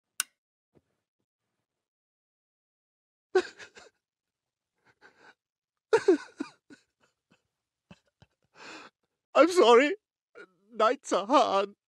"I'm sorry, nights are hard."
Yes, this has been a real issue i've been facing as of last month. Whenever I go to sleep, I cry myself to sleep. Mainly because I'm still mentally scarred from what happened.

**Also sorry for not posting, I'll get onto posting again soon.**